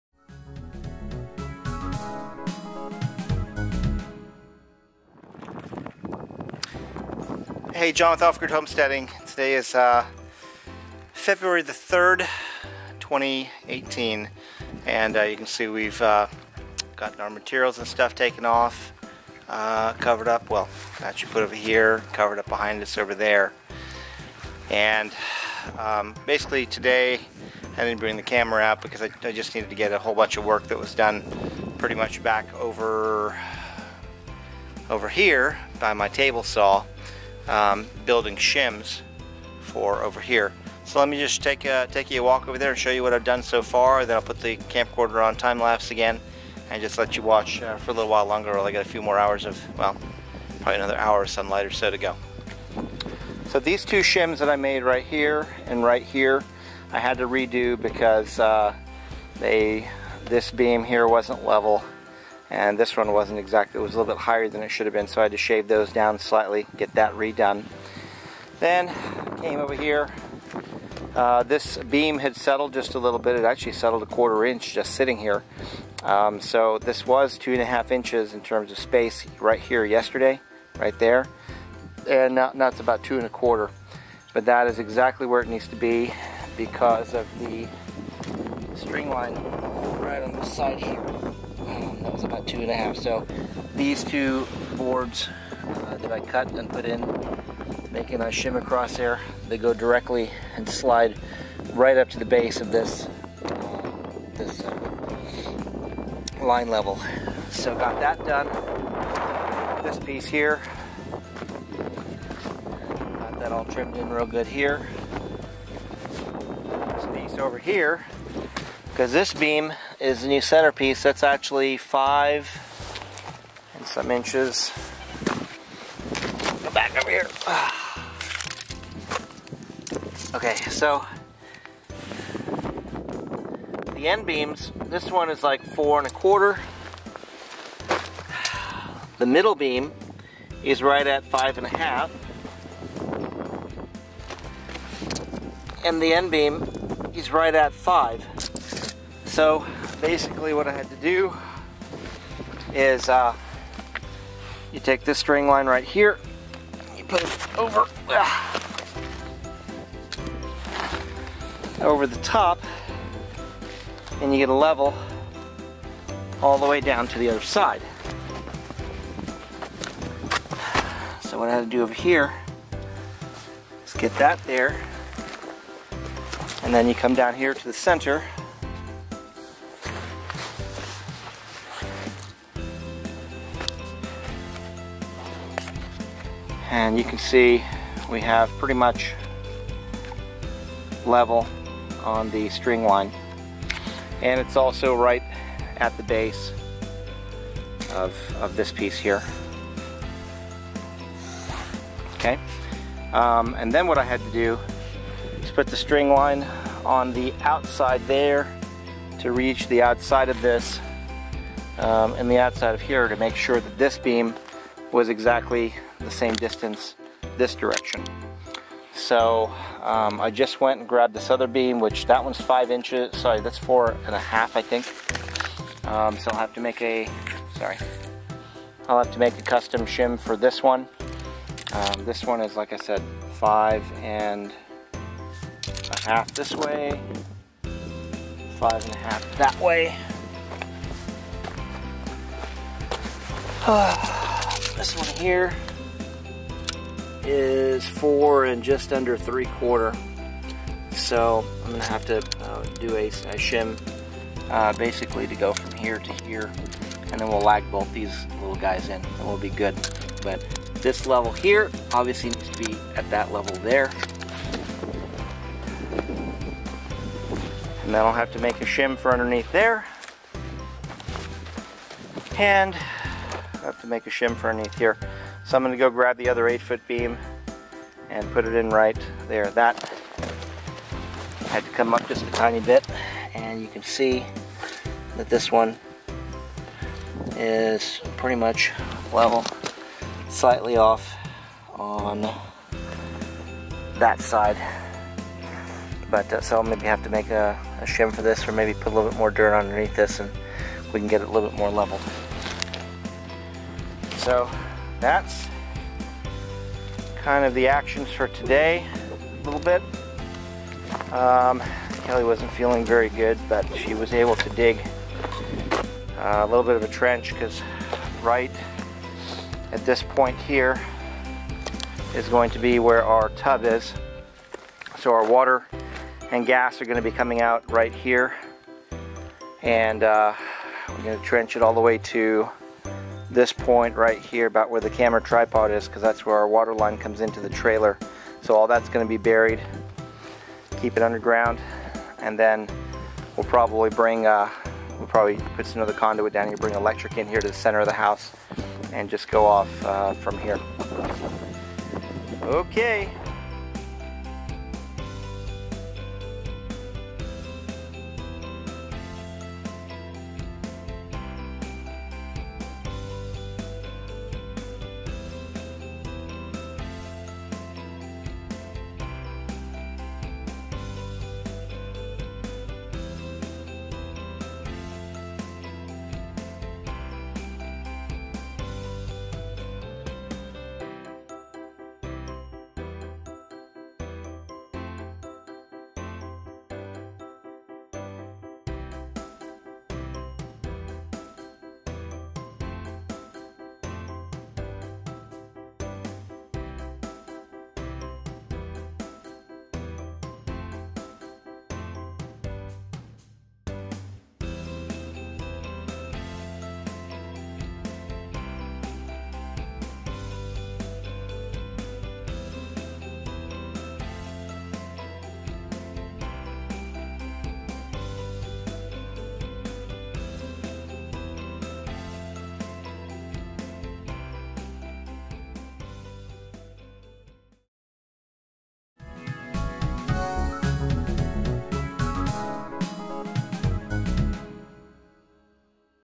OGH - Setting CENTER beams and Shimming Feb 3, 2018
Today I worked by myself - Ed and Family were off, will be back in 2 days on Monday.  I was able to redo and set the center beam.